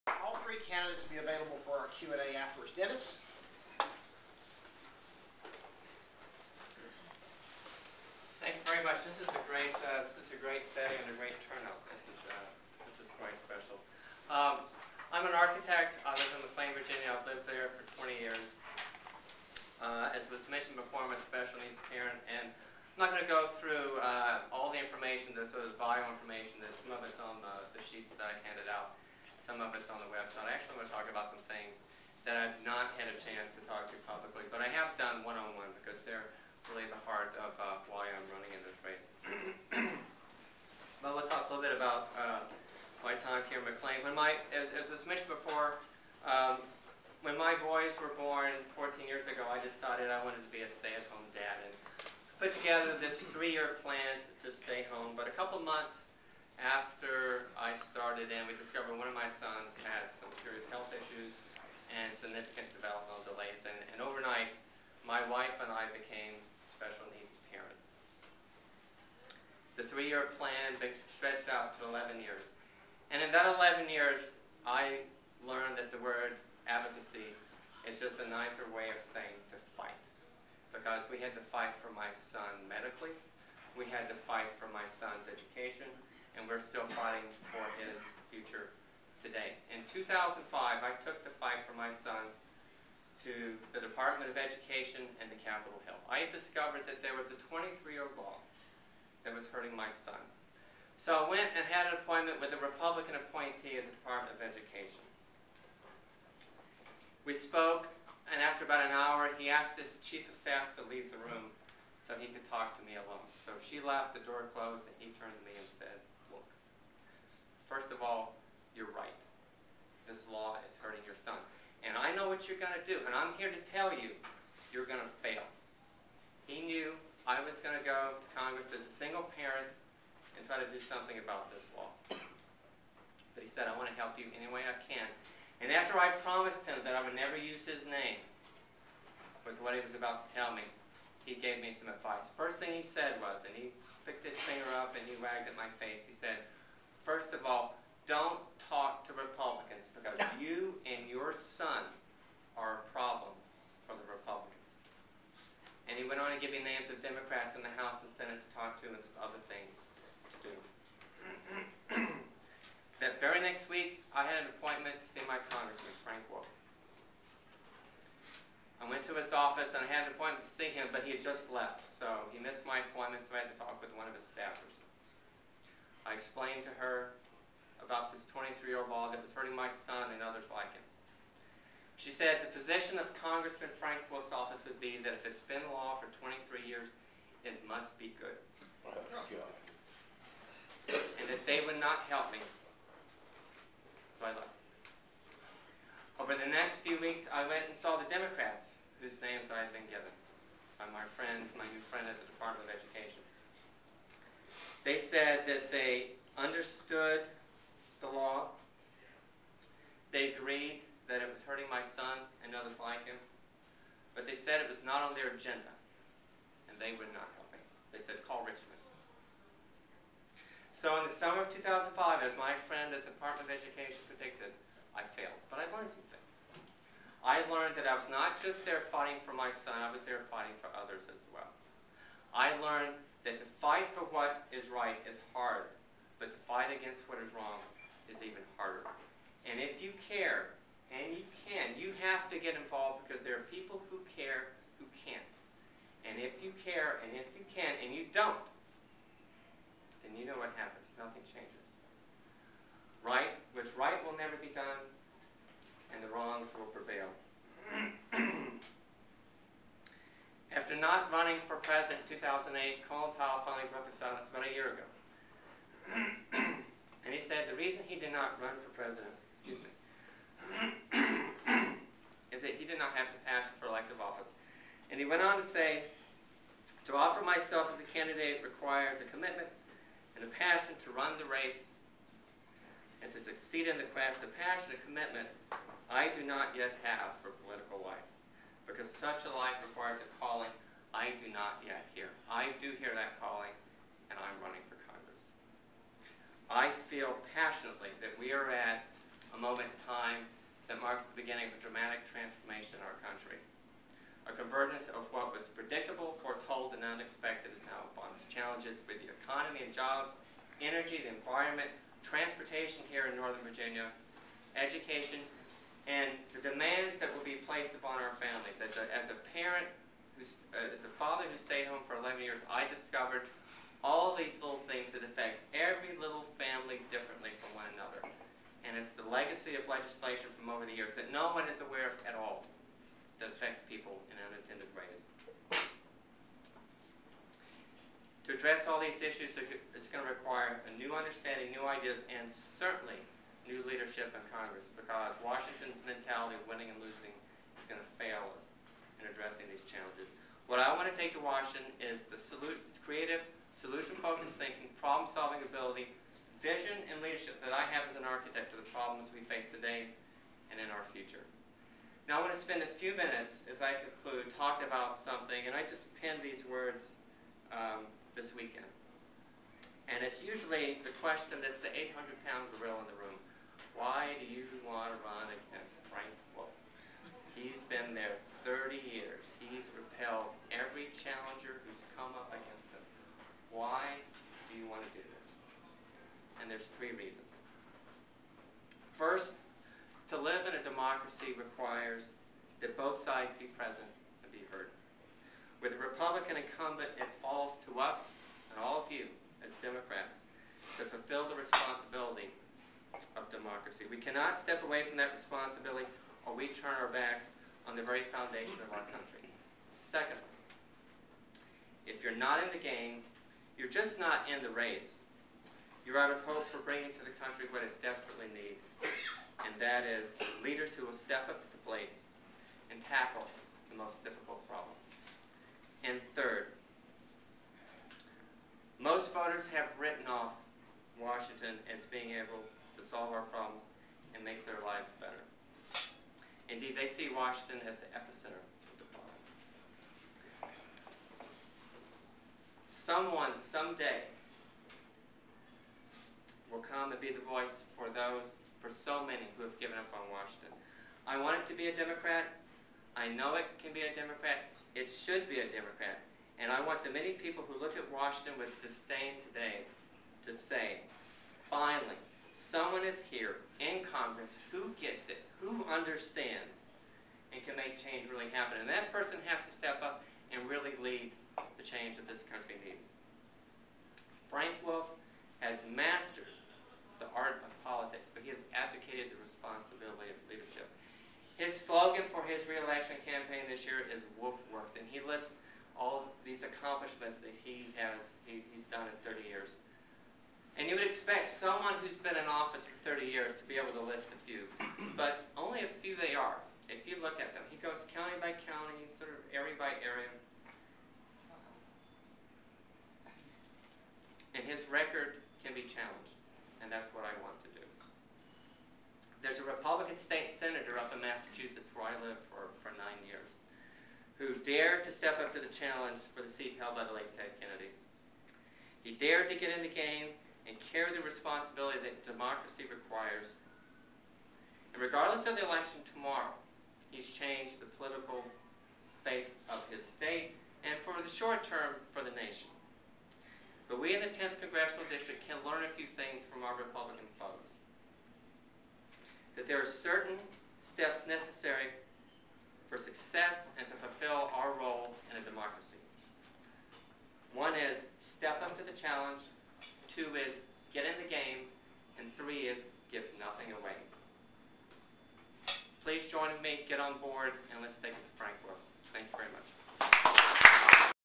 Dennis Findley Speaks at Dulles Area Democrats
10th Congressional District Congressional candidate Dennis Findley speaks at the Dulles Area Democrats breakfast on 1/18/10.